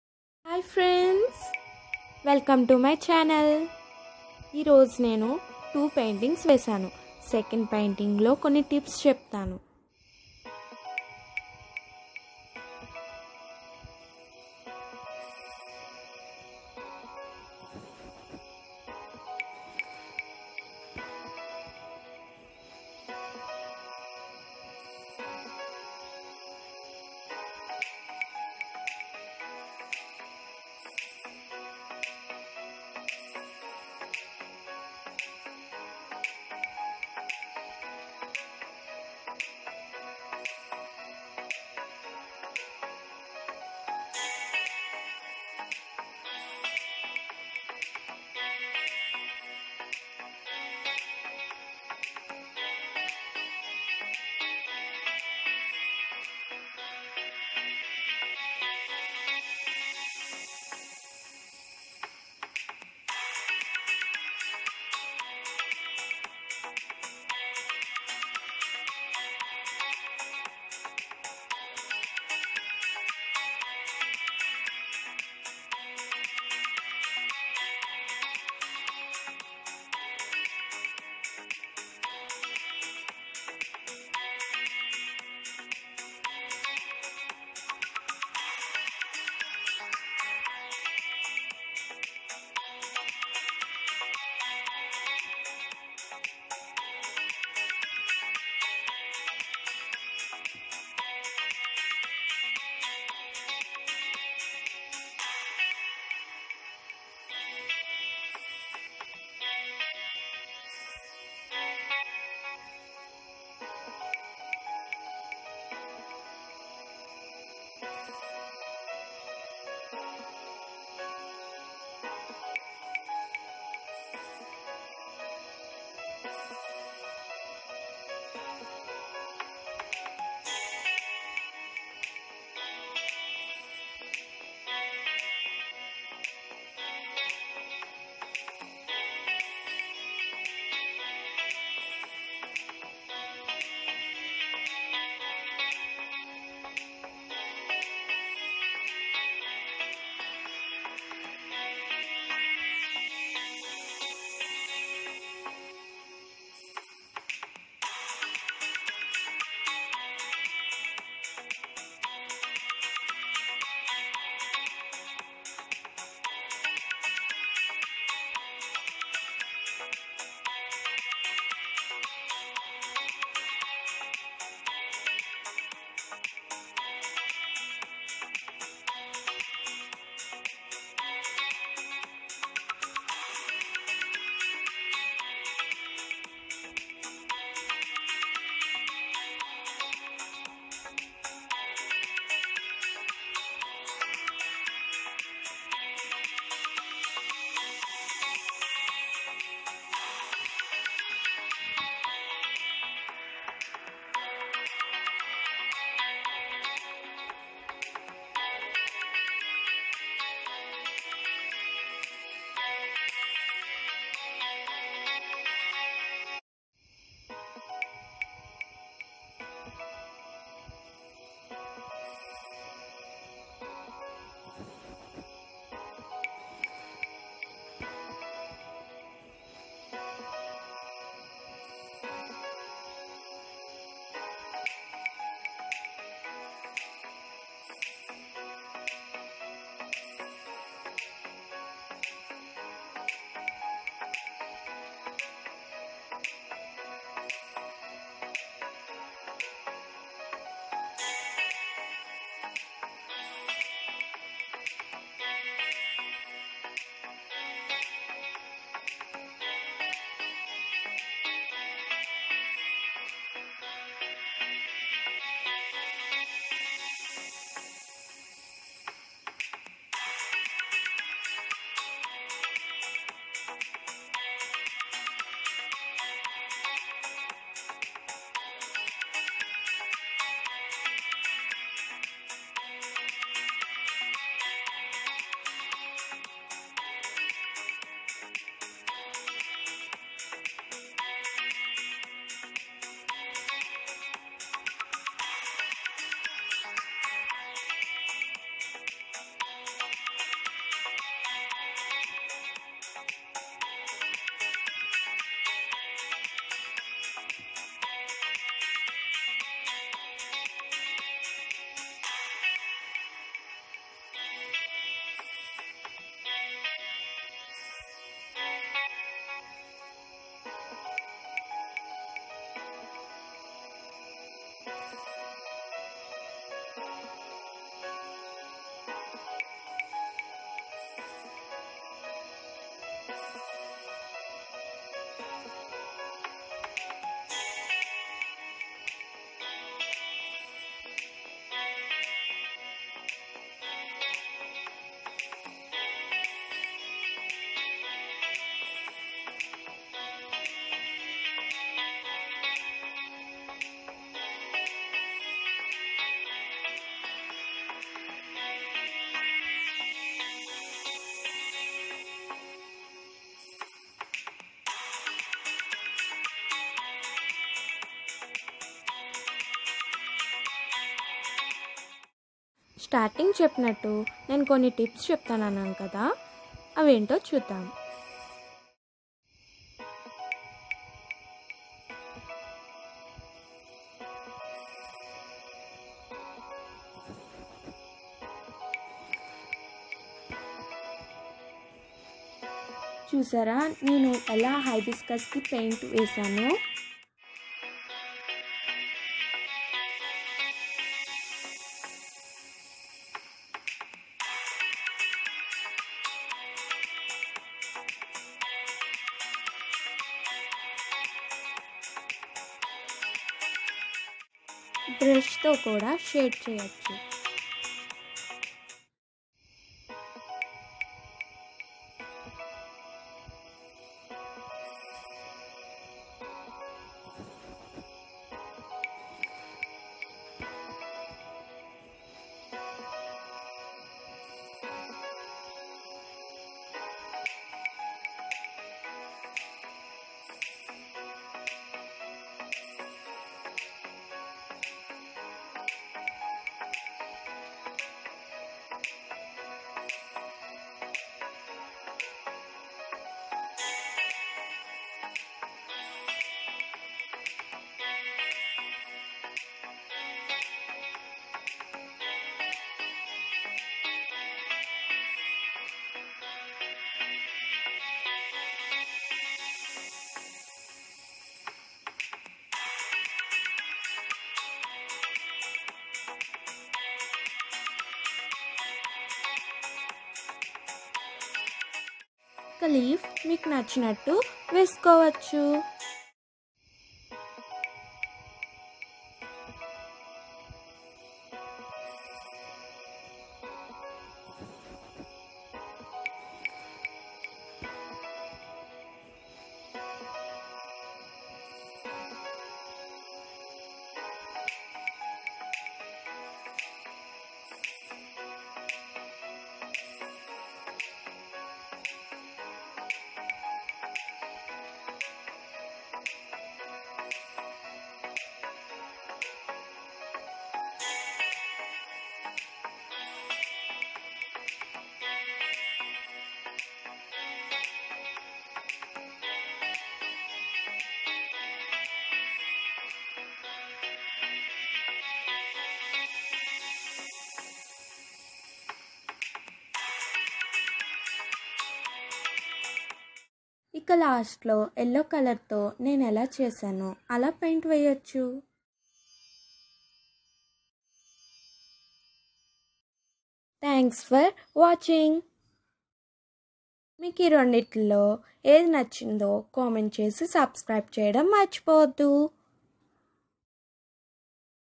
Landscape Oil painting ,Hibiscus Acrylic paint -Tejaswi
Painting   - 7-8 hours
Editing      - 8-9 hours

Hey guys, today I've painted two drawings. One is hibiscus by acrylic paint and the other is landscape by oilpaint. I liked both. Tell me your favourite painting in the comments section.